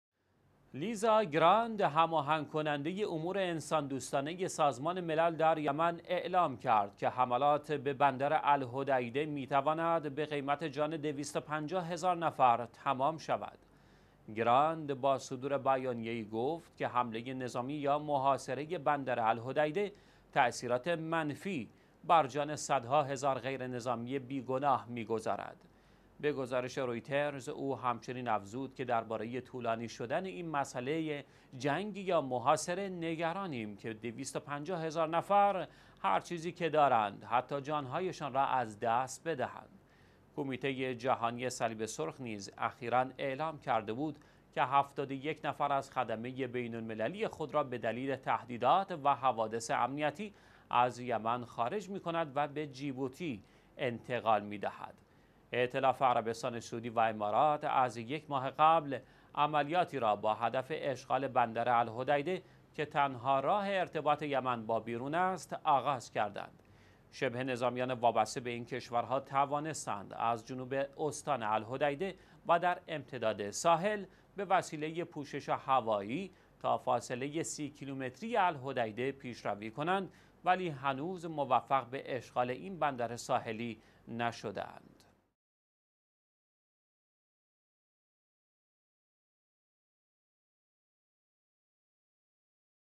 هشدار سازمان ملل در باره حملات عربستان و امارات به غرب یمن
«لیزا گراند» هماهنگ‌کننده امور انسان‌دوستانه سازمان_ملل در یمن اعلام کرد که حملات به بندر «الحدیده» می‌تواند به قیمت جان ۲۵۰ هزار نفر تمام شود.
گراند، با صدور بیانیه‌ ای گفت: «حمله نظامی یا محاصره بر بندر الحدیده تاثیرات منفی بر جان صدها هزار غیر نظامی بی‌گناه می‌گذارد».
به گزارش «رویترز» وی همچنین افزود: «در موردی که این مسئله (جنگ یا محاصره) ‌طولانی شود، نگران هستیم که ۲۵۰ هزار نفر هر چیزی که دارند حتی جان‌هایشان را از دست بدهند».
کمیته جهانی صلیب_سرخ نیز اخیرا اعلام کرده بود که ۷۱ نفر از خدمه بین‌_المللی خود را به دلیل تهدیدات و حوادث امنیتی، از یمن خارج کرده و به جیبوتی منتقل می‌کند.
ائتلاف عربستان سعودی و امارات از یک ماه پیش عملیاتی را با هدف اشغال بندر الحدیده که تنها راه ارتباط یمن با بیرون است، آغاز کردند.
شبه‌نظامیان وابسته به این کشورها توانستند از جنوب استان الحدیده و در امتداد ساحل با پوشش هوایی، تا نزدیکی الحدیده (فاصله ۳۰ کیلومتری) پیشروی‌ کنند، ولی هنوز نتوانسته اند این بندر ساحلی را اشغال نمایند.